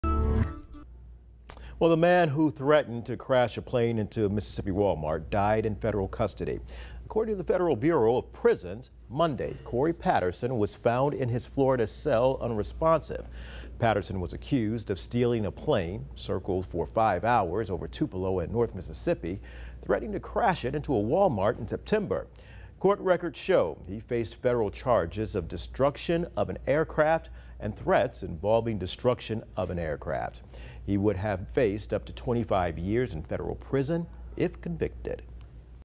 Cory Patterson, who threatened to crash a plane into a Tupelo Walmart, dies in federal custody
According to the Federal Bureau of Prisons, 29-year-old Cory Patterson was found unresponsive in his cell Monday.